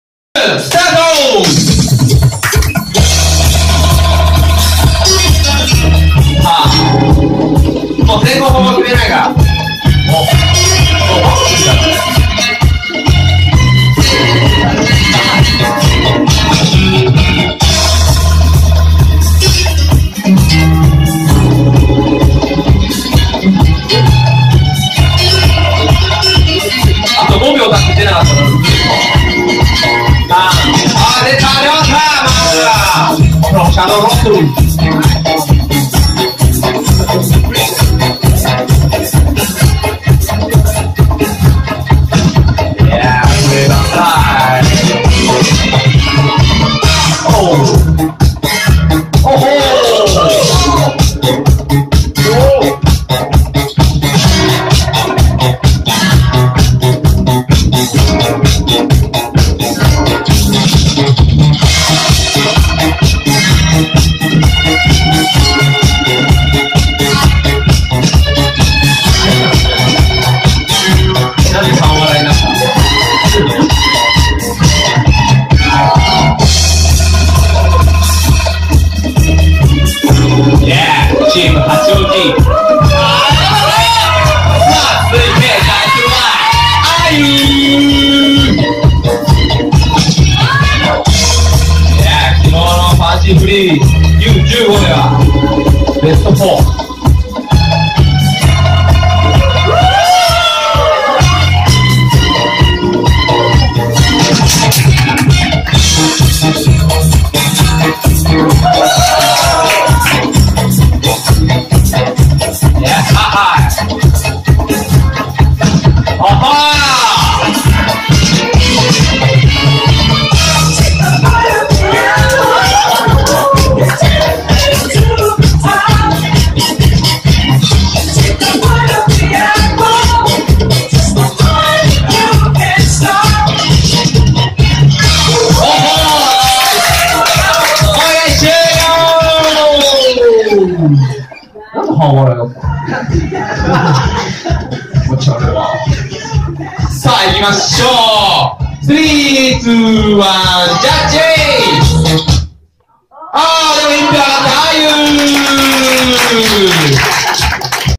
BEST16 AYU vs RYOTA ~TNC FreeStyle Battle~
2014/02/23 TNC Free Style Battle @TNC
Winner AYU
JUDGE [UEN] [ZOOKY]
MC [FU-TA] DJ L.A.B.